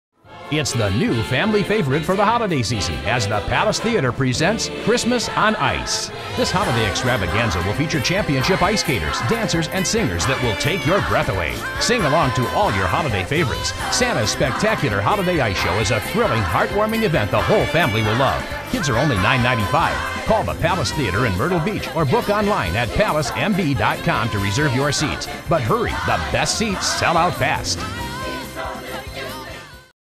PalaceTheatre - Christmas on Ice Promotional spot
PalaceTheatre - Christmas on Ice Promotional spot.  Shot over the course of 2 days to capture the magic of this show in Myrtle Beach, S.C.